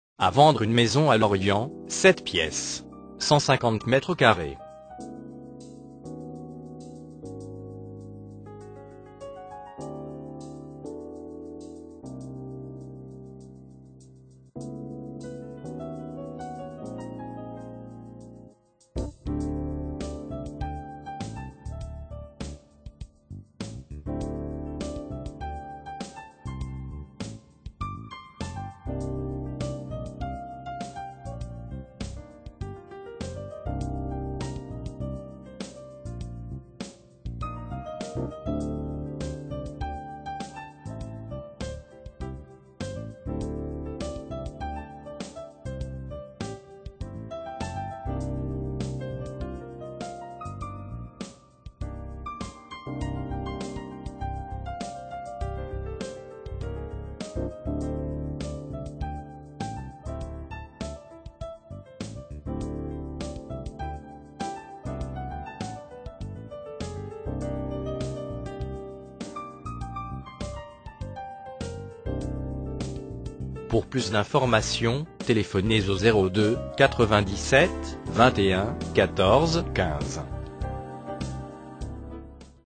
Achat Vente Maison LORIENT 56100 - 150 m2
A vendre à LORIENT une maison de 150 m2.

Annonce immobilière : Achat Vente Maison LORIENT 56100 Morbihan